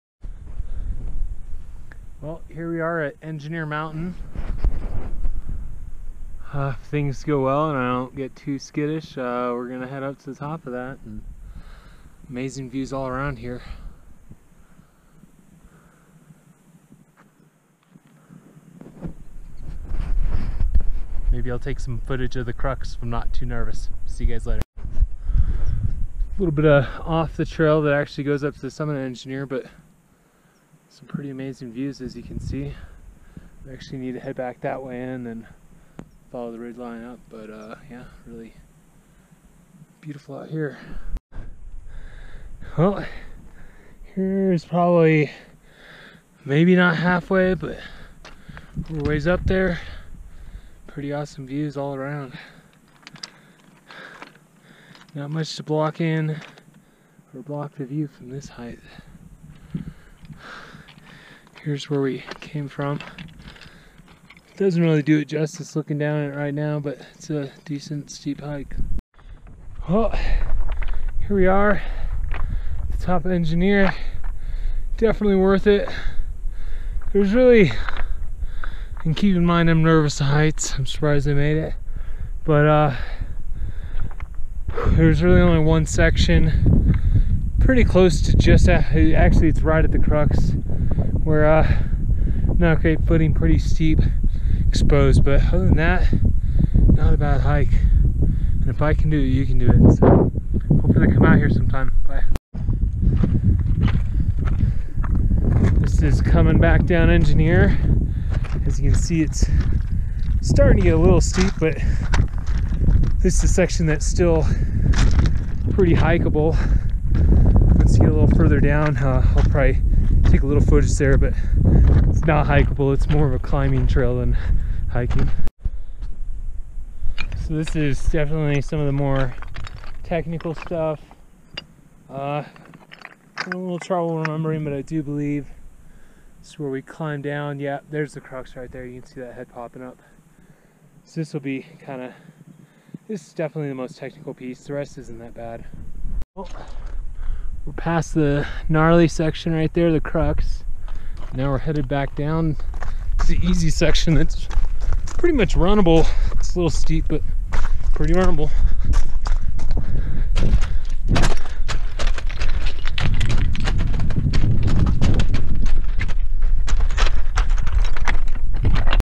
EngineerMountain
Hike up Engineer Mountain.  Amazing hike with awesome views.  To see information on length/elevation gain on this hike see the following Strava activity: